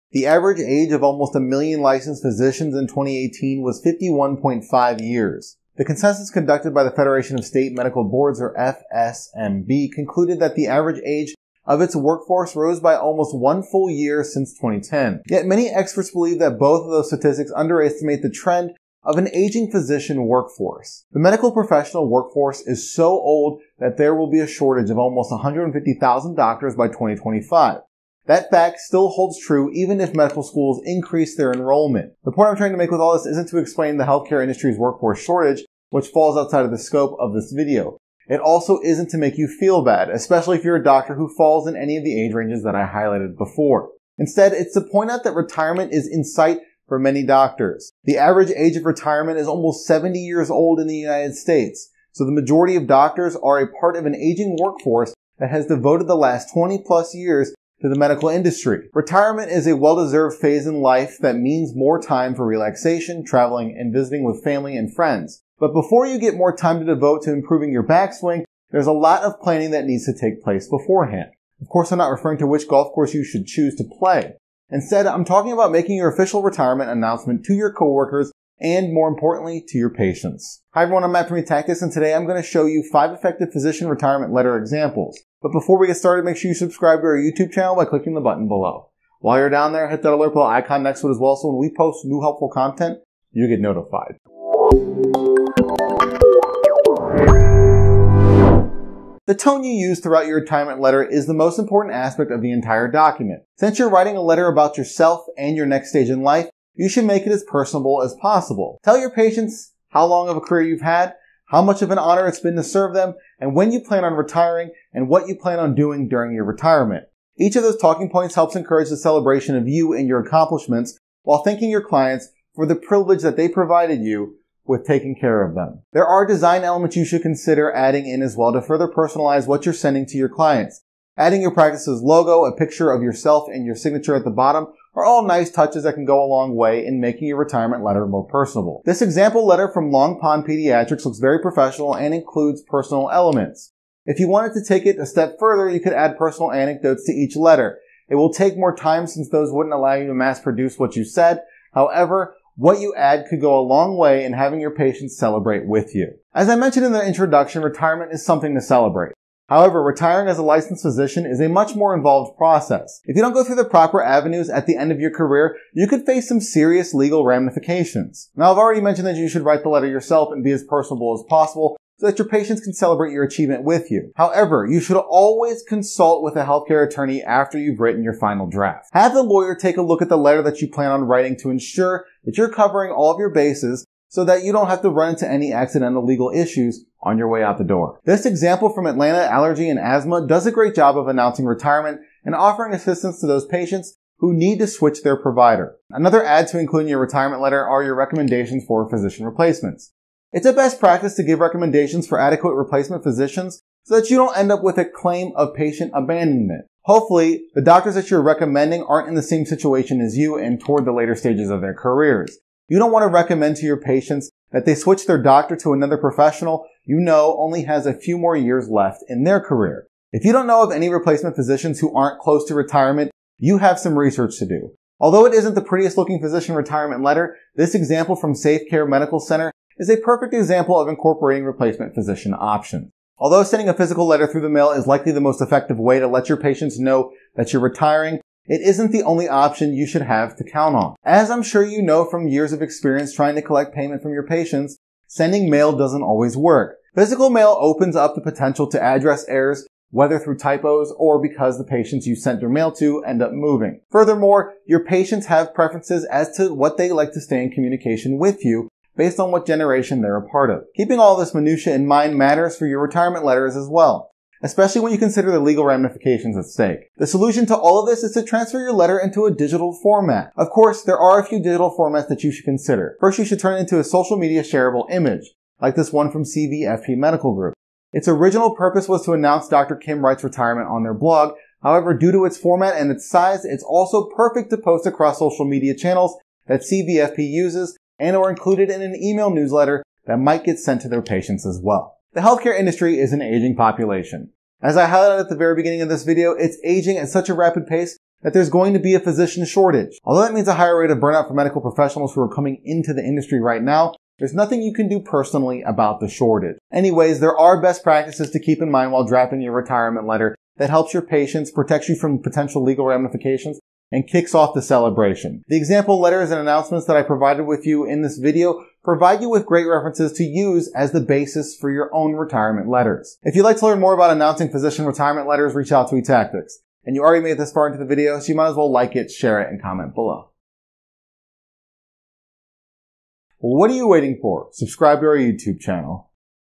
5 Physician Retirement Letter Examples
The average age of the 985,000 licensed physicians in 2018 was 51.5 years. The biennial census conducted by the Federation of State Medical Boards (FSMB) concluded that the average age of its workforce rose by almost 1 full year since 2010.

Yet, many experts believe that both of those statistics underestimate the trend of an aging physician workforce.

The medical professional workforce is so old that there will be a shortage of almost 150,000 doctors by 2025. That fact still holds even if medical schools increase their enrollment.

The point I’m trying to make with all of this isn’t to explain the healthcare industries’ workforce shortage, which falls outside of the scope of this blog. It also isn’t to make you feel bad, especially if you’re a doctor who falls in any of the age ranges I highlighted.

Instead, it’s to point out that retirement is in sight for many doctors. The average age of retirement is almost 70 years old in the United States. So, the majority of doctors are a part of an aging workforce that has devoted the last 20 plus years to the medical industry.

Retirement is a well-deserved phase in life that means more time for relaxation, traveling and visiting with family and friends. But before you get more time to devote to improving your backswing, there’s a lot of planning that needs to take place.

I’m not referring to planning on which golf courses to play at. Instead, I’m talking about making your official retirement announcement to your coworkers and, more importantly, to your patients.

The tone you use throughout your retirement letter is the most important aspect of the entire document. Since you’re writing a letter about yourself and your next stage in life, you should make it as personable as possible. Tell your patients; how long of a career you’ve had, how much of an honor it has been to serve them, when you plan on retiring, and what you plan on doing during retirement.

Each of those talking points helps encourage the celebration of you and your accomplishments while thanking your clients for the privilege to provide them care. They also help ease the transition all of your patients will have to go through in selecting a new provider.

There are design elements you should consider adding in as well to further personalize what you’re sending to your clients.

Adding your practice’s logo, a picture of yourself and your signature at the bottom are all nice touches that can go a long way in making your retirement letter more personable.

This example letter from Long Pond Pediatrics looks very professional and includes personal elements.

If you wanted to take it a step further, you could add personal anecdotes to each letter. It will take more time since those wouldn’t allow you to mass-produce what you send. However, what you add could go a long way in having your patient celebrate with you.

As I mentioned in the introduction, retirement is something to celebrate. However, retiring as a licensed physician is a much more involved process. If you don’t go through the proper avenues at the end of your career, you could face some serious legal ramifications.

Throughout the rest of this blog post, I’ll mention how you should write your announcement letter yourself and be as genuine as possible so that your patients can celebrate your achievement with you.
However, you should always consult with a healthcare attorney after you’ve written your final draft.

Have the lawyer take a look at the letter you plan on writing to ensure that you’re covering your bases so that you don’t run into any accidental legal issues on your way out.

This example from Atlanta Allergy & Asthma does a great job of announcing retirement and offering assistance to those patients who need to switch their provider.

One of the must-adds to include in your retirement letter are your recommendations for physician replacements.

It’s a best practice to give recommendations for adequate replacement physicians so that you don’t end up with a claim of patient abandonment.
Hopefully, not all of those doctors are in the same situation as you and are toward the later stages of their careers. You don’t want to recommend to your patients that they switch their doctor to another professional you know only to have them retire a year later. That’s a frustrating scenario.
If you don’t know of any replacement physicians who aren’t close to retirement, you have some research to do.